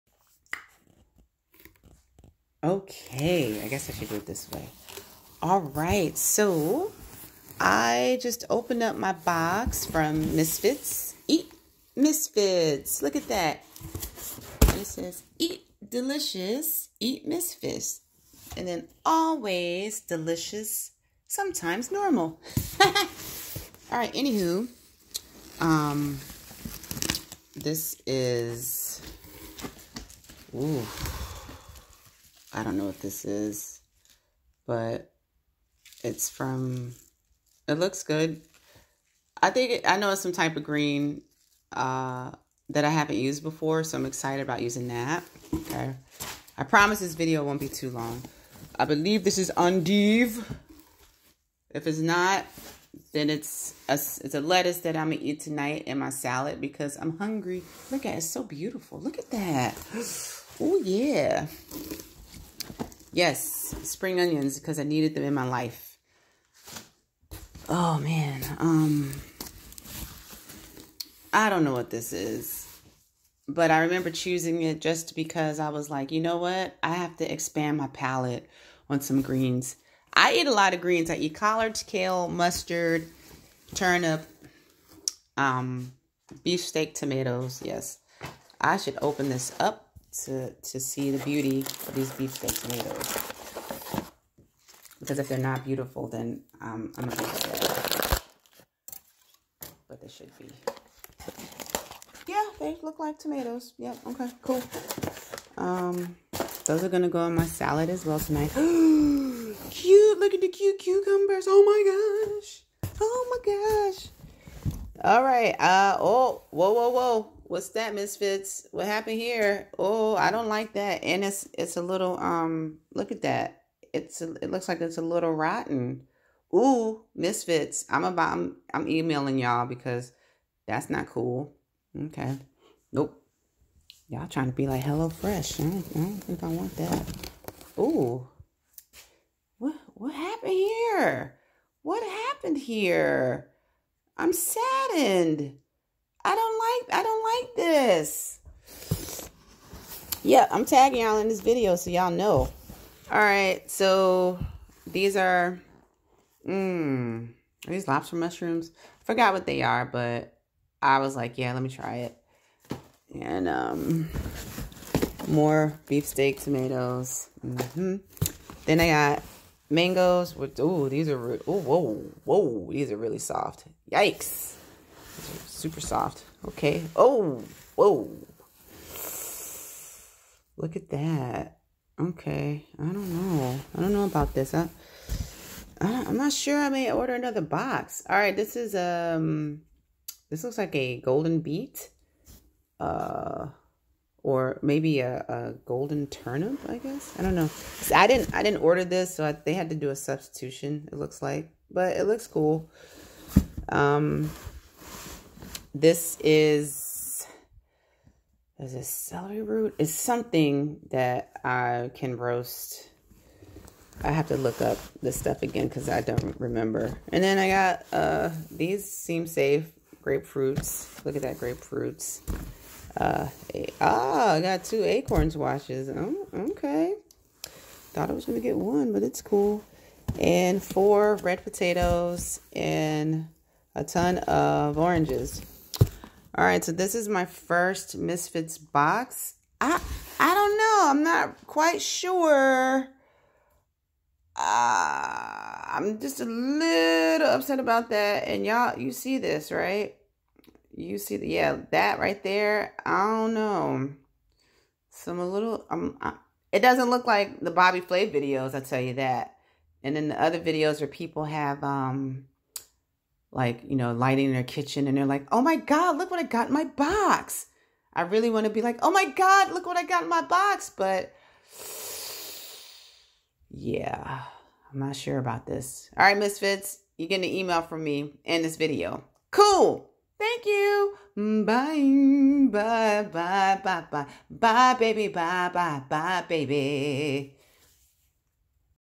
What's In My Box?: Misfits Veggie and Fruit Review
While I should know all the names of the veggies, I don't even though I put the box together upon ordering it.

A little disappointed that the cucumbers were crushed and the mangoes are REALLY ripe.

I made a salad.  It was tasty.   I ate it before capturing the beauty.🤣